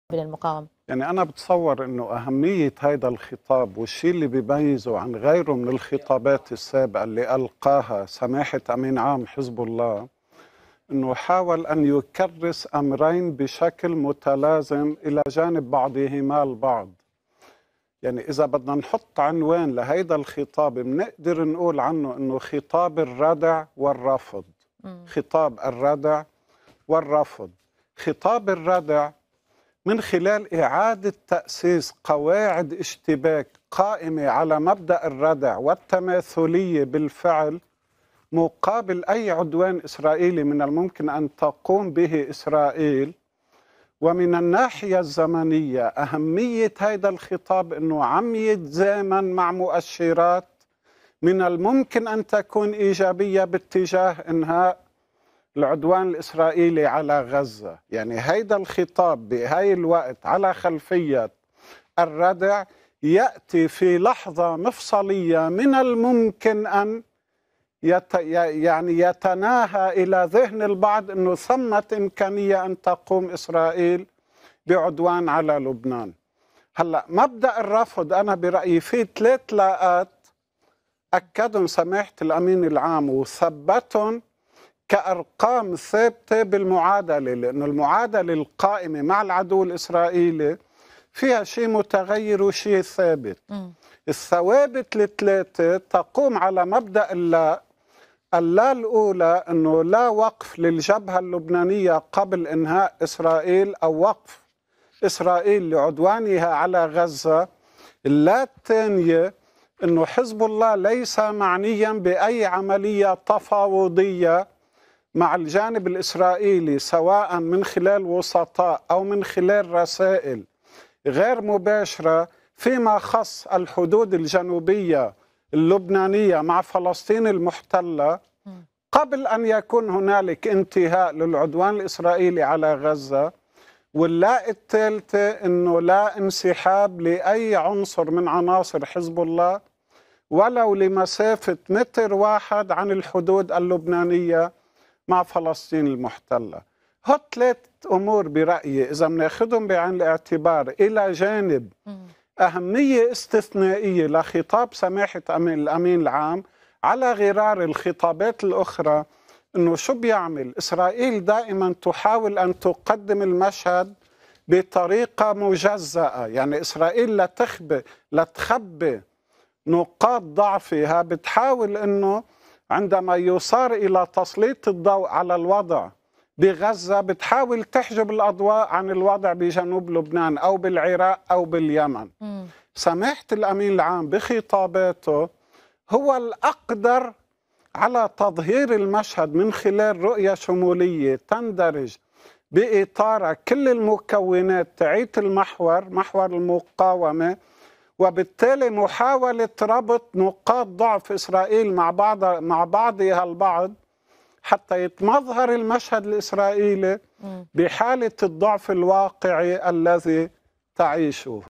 يمكن وصف خطاب السيد نصرالله اليوم بأنه خطاب الردع والرفض
الخبير في الشؤون الإقليمية حبيب فياض